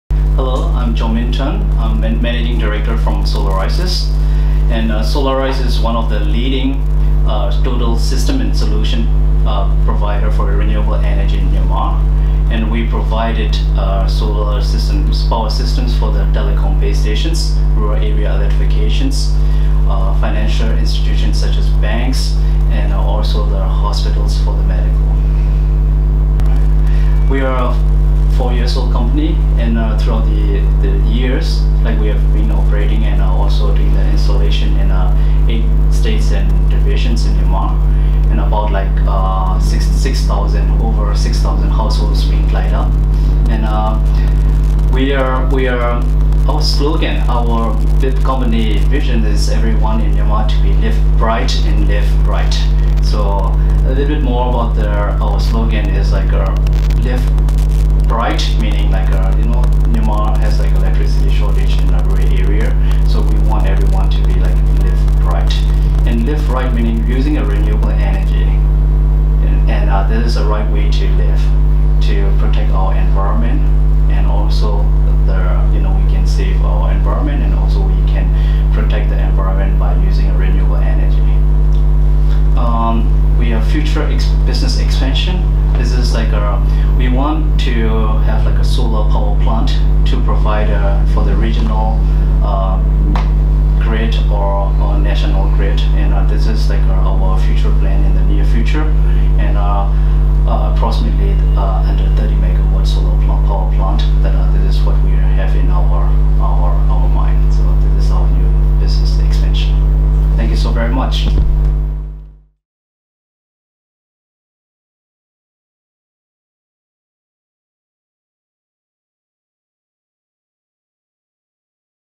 INTERVIEWS: Kyaw Min Tun - SolaRiseSys
Taken during the Myanmar Business Model Workshop - 30 May 2014